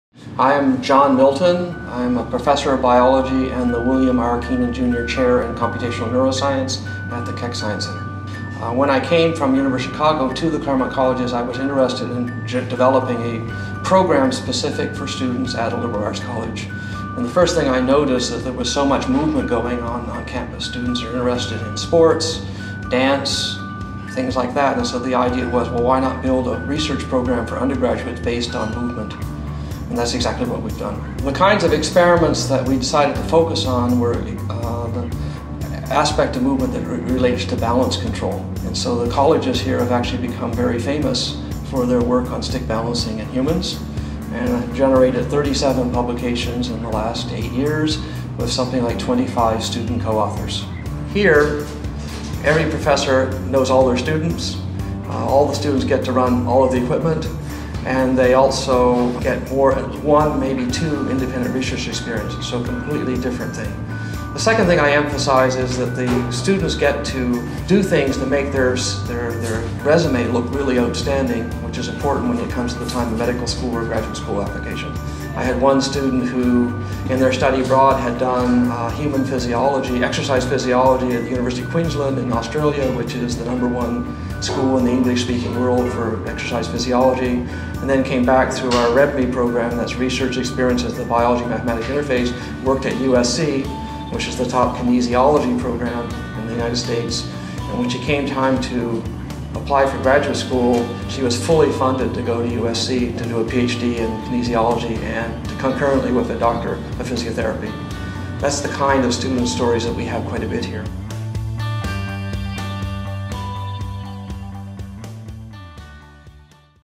Interview with William R. Kenan Jr. Professor of Computational Neuroscience John Milton
William R. Kenan Jr. Professor of Computational Neuroscience John Milton tells us why he thoroughly enjoys teaching at the W. M. Keck Science Department.